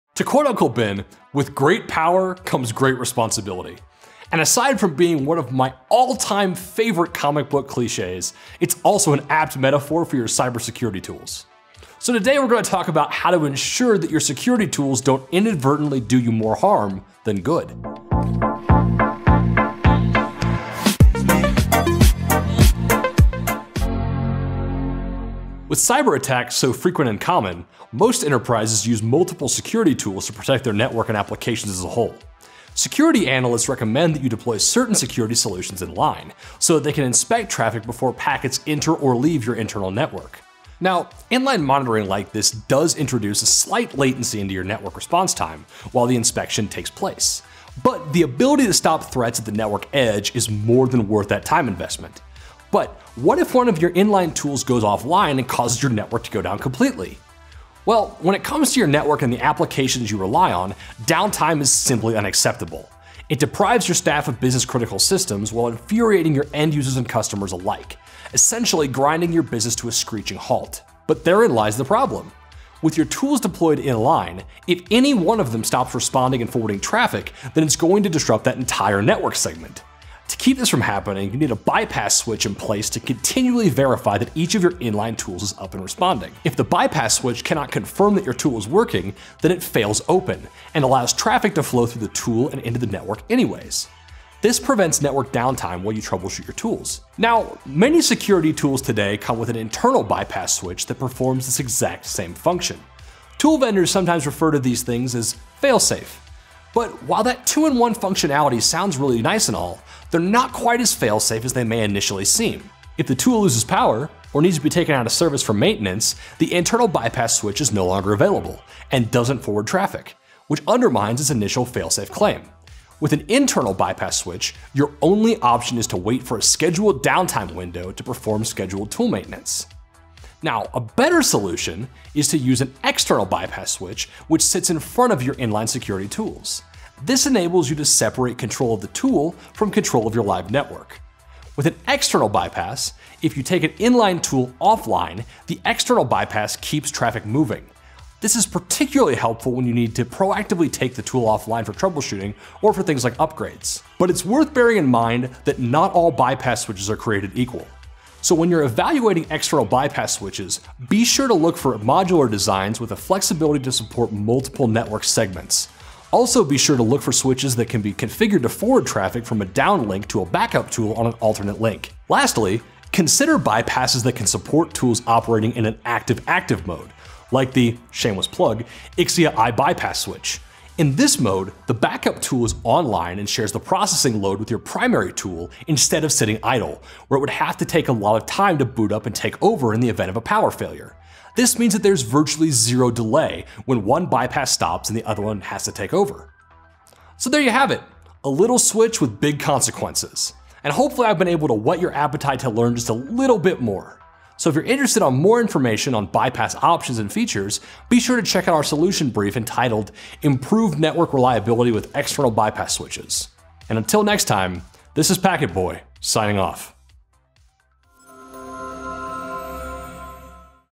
What is a Bypass Switch? | Tech Tips with Packet Boi
What is a bypass switch, and what does it do? Basically, it keeps your security tools from doing more harm than good.

To quote Uncle Ben, “With Great Power Comes Great Responsibility.” And aside from being one of my favorite comic-book clichés, it’s also an apt metaphor for your cyber security tools. So, today we’re going to talk about how to ensure your security tools don’t inadvertently do more harm than good.

With cyberattacks so frequent and common, most enterprises use multiple security tools to protect their network and applications. Security analysts recommend you deploy certain security solutions inline, so they can inspect traffic before packets enter or leave your internal network.

Now, inline monitoring like this does introduce a slight latency in network response time while the inspection takes place, but the ability to stop threats at the network edge is more than worth it.

But what if one of your inline tools goes offline and causes your network to go down completely?

When it comes to your network and the applications you rely on, downtime is simply unacceptable. It deprives your staff of business-critical systems while infuriating your end-users and customers alike—essentially grinding your business to a screeching halt. But therein lies the problem: with your tools deployed inline, if any one of them stops responding and forwarding traffic, then it’s going to disrupt that network segment.

To keep this from happening, you need a bypass switch in place to continually verify that each of your inline tools is up and responding.

If the bypass switch cannot confirm your tool is working, then it ‘fails open’ and allows traffic to flow through the tool and into the network anyway. This prevents network downtime while you troubleshoot your tool.

Now, many security tools today come with an internal bypass switch that perform this exact same function. Tool vendors sometimes refer to these as “fail safe.” But while that 2 for 1 functionality sounds really nice and all, they’re not quite as fail safe as they may initially seem. If the tool loses power or needs to be taken out of service for maintenance, the internal bypass simply won’t function, undermining it’s fail-safe utility and preventing you from forwarding traffic. With an internal bypass, your only option is to wait for a scheduled downtime window to perform tool maintenance.

A better solution is to use an external bypass switch in front of your inline security tools. This enables you to separate control of the tool from control of your live network.

With an external bypass, if you take an inline tool offline, the external bypass keeps traffic moving. This is particularly helpful when you need to proactively take the tool offline for troubleshooting or upgrades.

Granted, bypass switches aren’t created equal. So when you’re evaluating external bypass switches, look for modular designs with the flexibility to support multiple network segments. Also be sure to look for switches than can be configured to forward traffic from a down link to a backup tool on an alternate link.

Lastly, you should consider bypasses that can support tools operating in ‘active-active’ mode, like Ixia bypass switches! In this mode, the backup tool is online and shares the processing load with your primary tool instead of sitting idle, where it would have to boot up to take over in the event of a failure. This means that there is virtually no delay if one bypass stops and the other must take over.

So there you have it—a little switch with big consequences. And hopefully I’ve been able to whet your appetite to learn just a bit more.  So if you’re looking for more information on bypass options and features, be sure to check out our solution brief called “Improve Network Reliability with External Bypass Switches.”

Until next time, this is packet boi, signing off!